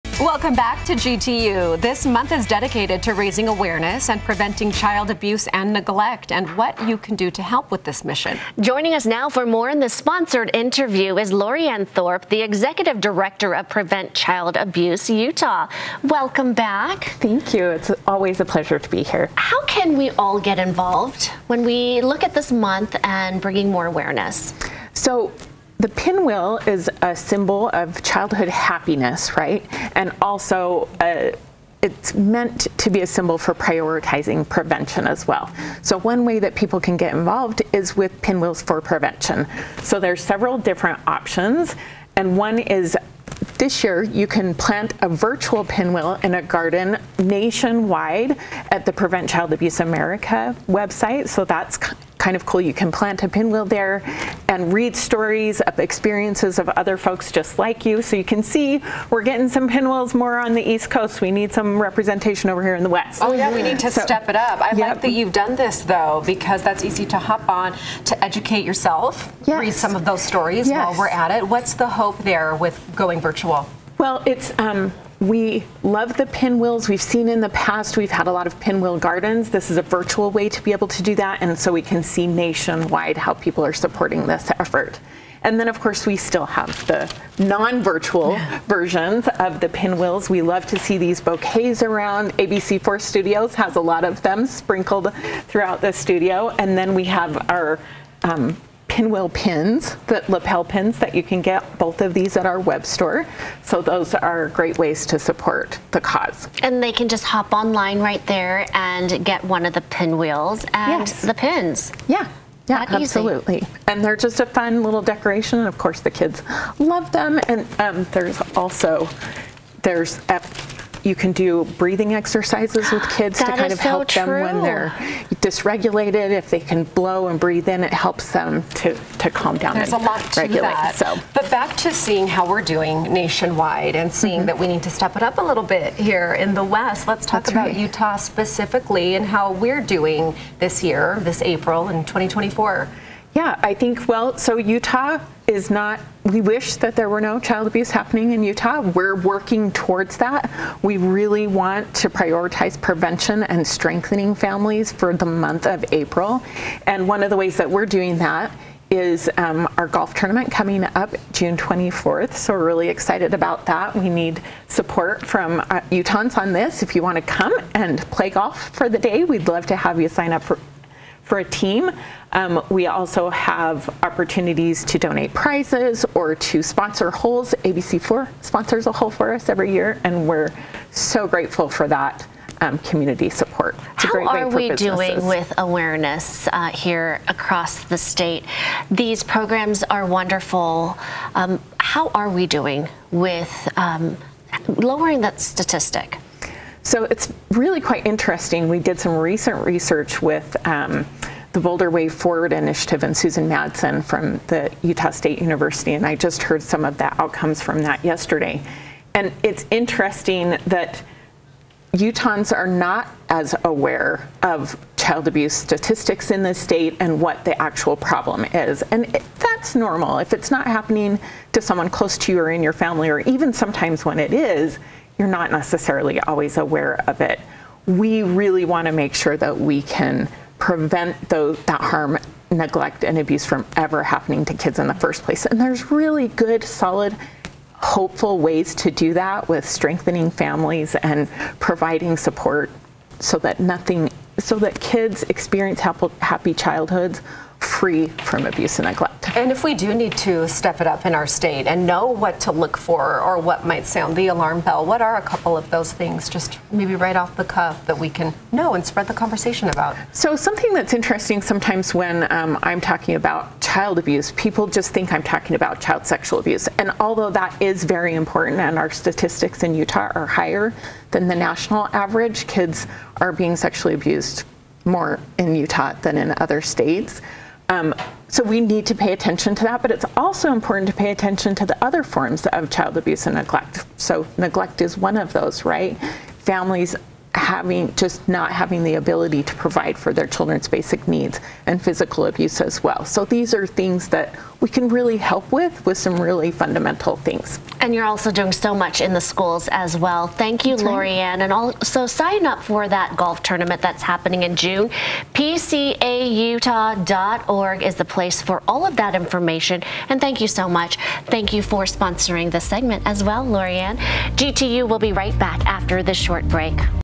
PCAU Recognizes Child Abuse Prevention Month
Help Prevent Child Abuse Utah spread awareness and education across Utah to help keep kids safe and nurture stronger families and children everywhere. April is Child Abuse Awareness Month - visit PCAUtah.org to see how you can help. Or shop online at shop.pcautah.org now to show your support.